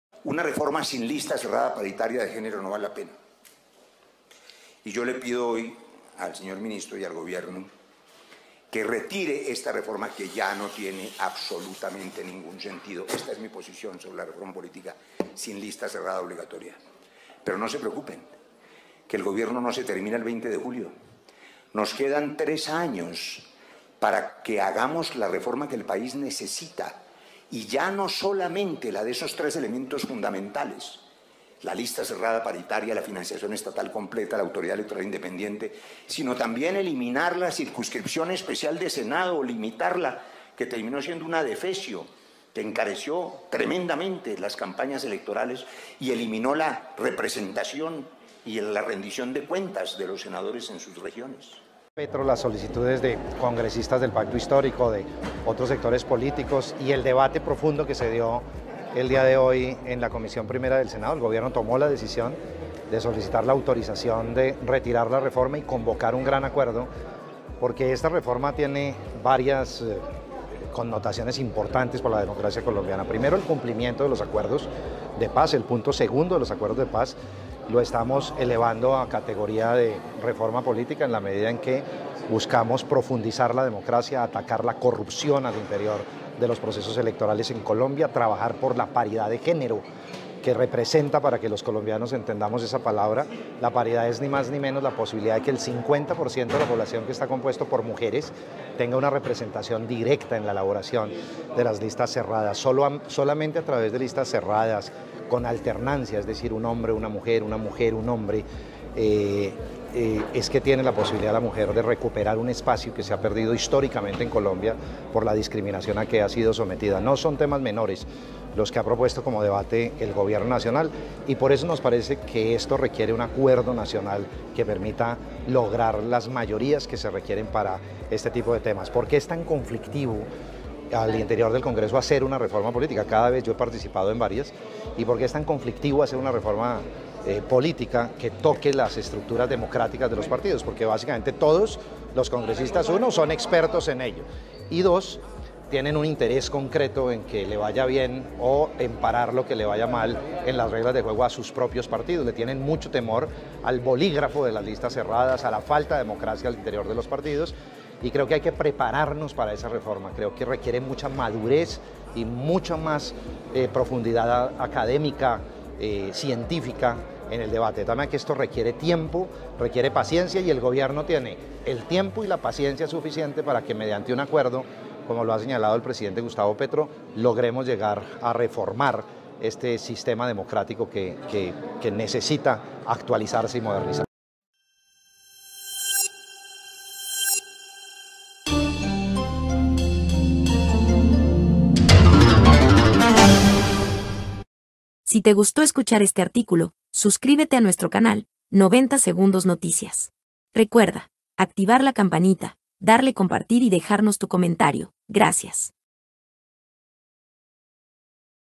🎥Declaración a medios del MinInterior Alfonso Prada sobre la Reforma Política 👇
El Gobierno Nacional convocará un gran acuerdo nacional para construir la reforma política que requiere el país y que representa un compromiso que se vinculó en los acuerdos de paz.

Así lo confirmó este jueves el Ministro del Interior, Alfonso Prada, quien aseguró que “atendiendo las instrucciones del Presidente Gustavo Petro, las solicitudes de congresistas del Pacto Histórico y otros sectores políticos, y el debate profundo que se dio hoy en la Comisión Primera del Senado, el Gobierno tomó la decisión de solicitar la autorización de retirar la reforma y convocar un gran acuerdo, porque esta reforma tiene varias connotaciones para la democracia colombiana”.

Al finalizar la sesión de la célula legislativa, el titular de la cartera explicó que esta reforma política se realizará en “cumplimiento de los acuerdos de paz, el punto segundo”, el cual “lo estamos elevando a categoría de reforma política”.

Dijo que en esos acuerdos hubo un compromiso de avanzar en esta reforma política, pero “no se ha podido y tenemos la solidez para avanzar en una reforma lo mejor posible”.

Explicó que lo que se pretende es “profundizar la democracia, atacar la corrupción en los procesos electorales; la paridad de género”, de tal manera que las mujeres tengan “una participación directa en la conformación de las listas cerradas, con alternancia”.

“La idea es volver con ese gran acuerdo para construir la reforma que requiere Colombia”, anotó.👇👇
90 Segundos Noticias, Somos independientes, con neutralidad e imparciales, objetivos y honestos en su concepto estricto. Sin prejuicios, justos y equilibrados. ...